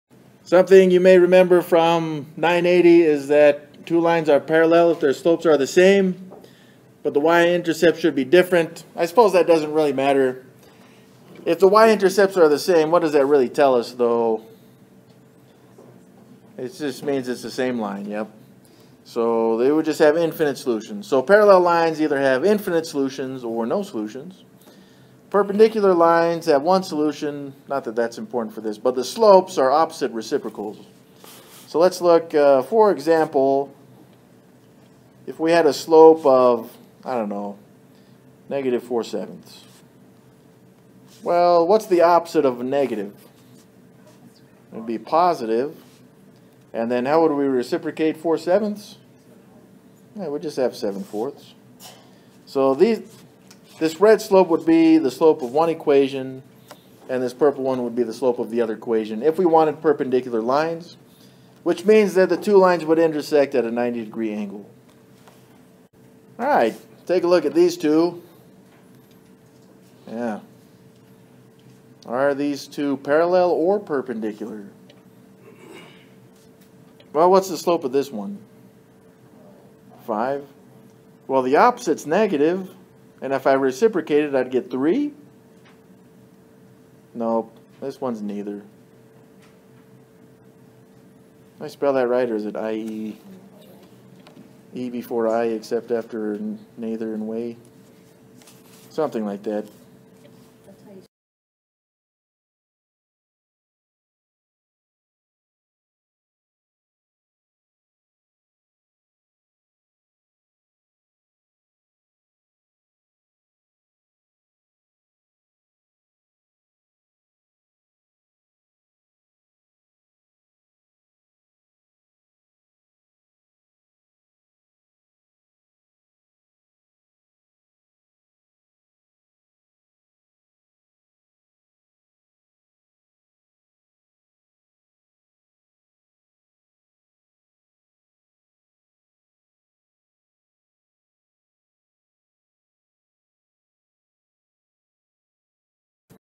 How to Determine if Two Straight Lines are Parallel or Perpendicular (a MATH 1010 Problem)
The problem:
Determine if the two lines are parallel or perpendicular
y=5x+4
y= -3x - 12

The solution:
Neither